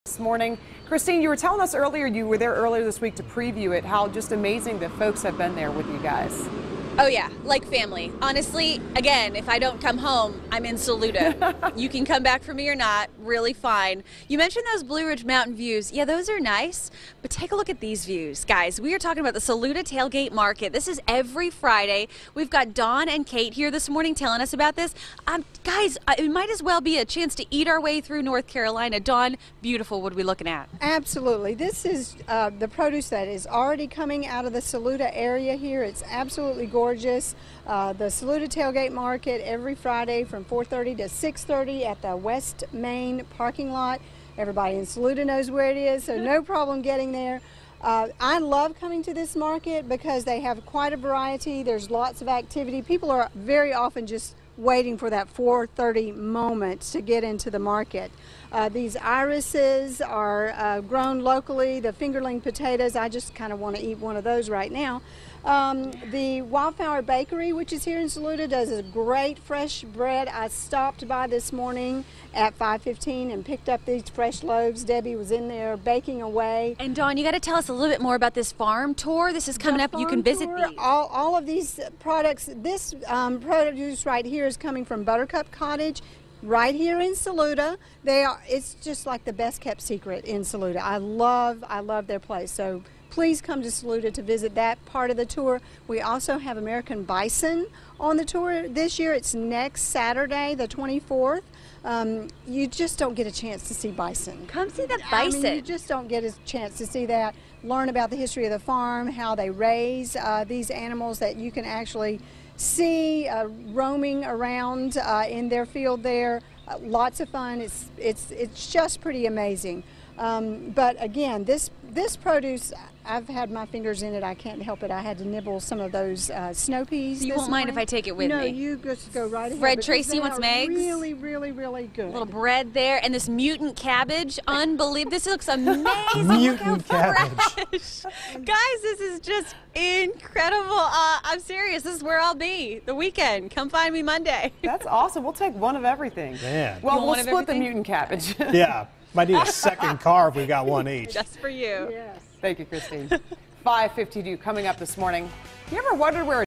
Hometown Spotlight Saluda, NC
Tucked away as you drive up the Saluda Grade and continue on into the Blue Ridge Mountains lies a small city called Saluda often frequented by visitors on weekends through the summers.